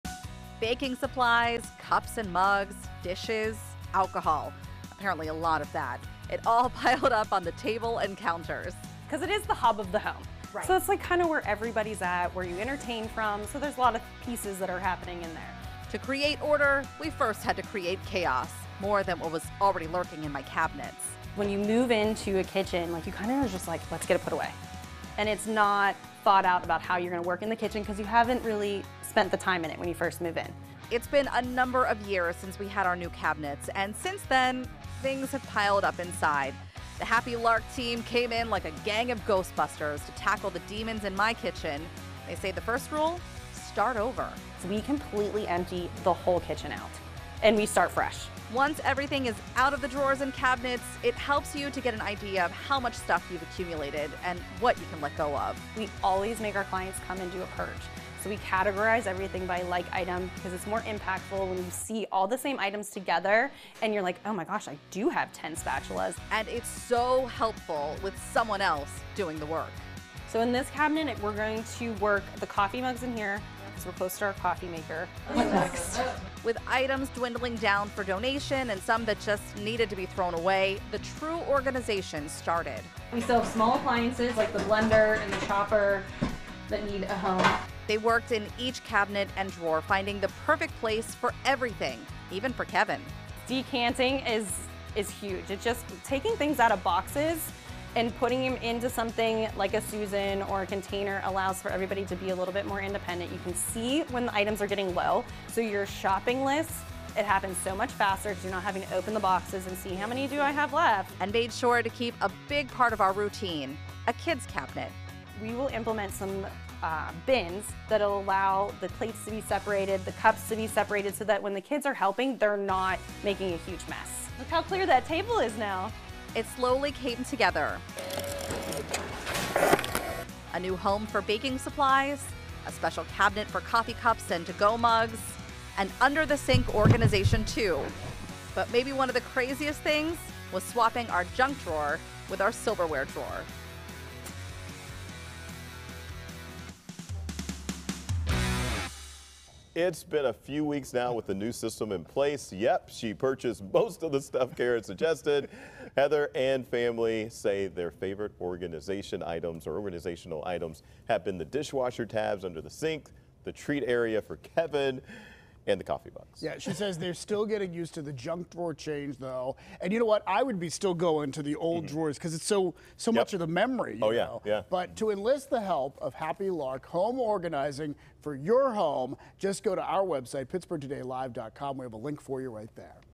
Getting a lesson in clutter-containment with Happy Lark Home Organizing
Heather took us back into her house to tackle mismatched tupperware with the help of Happy Lark Home Organizing.